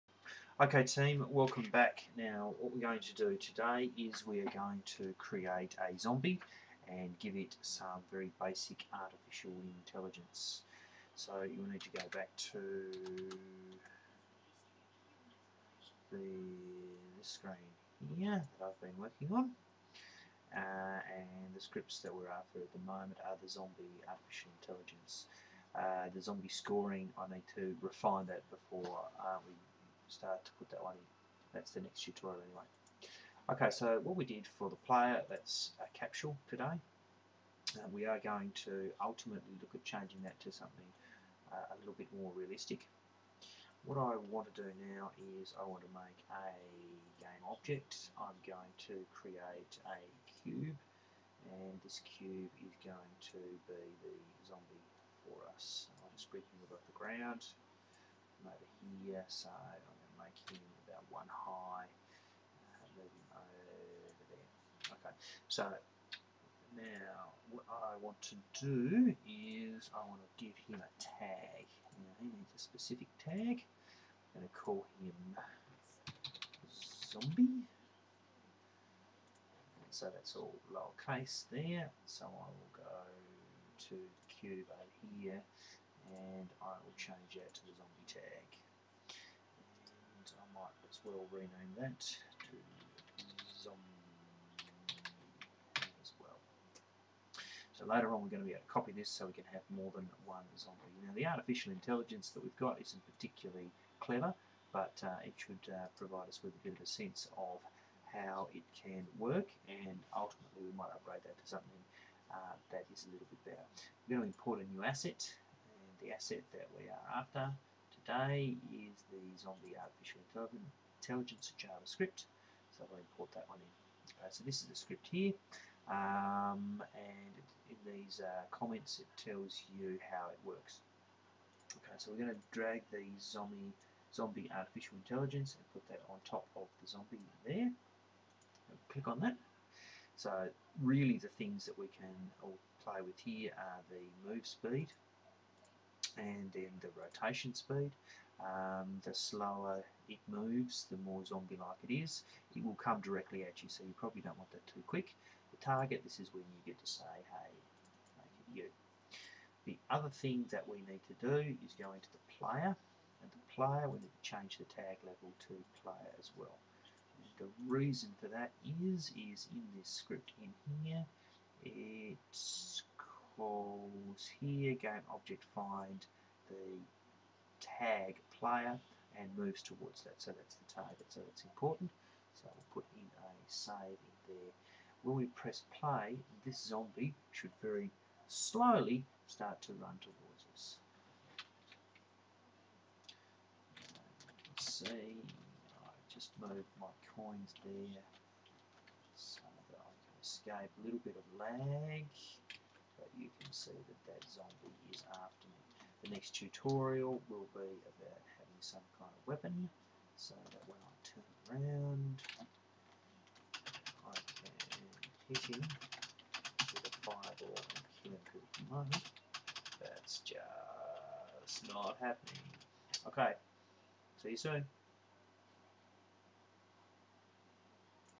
Unity 3D part 8, Zombie Artificial Intelligence
Lets add in some AI for our Zombie.
The Zombie AI script can be downloaded from here;